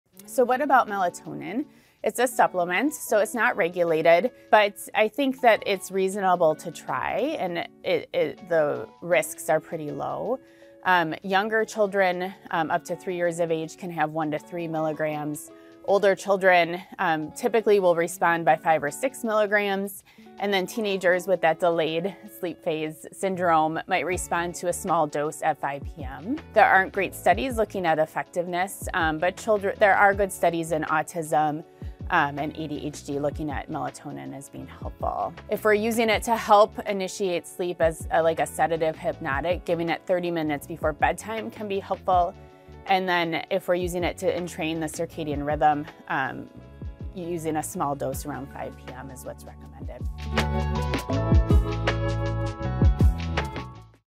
Using melatonin to help entrain circadian rhythm in children struggling with sleep
What about melatonin? Dr. Julie M. Baughn, a Pediatric Sleep Medicine Specialist at Mayo Clinic, explains that while it is an unregulated supplement, it can still be helpful to entrain the circadian rhythm in children struggling with sleep.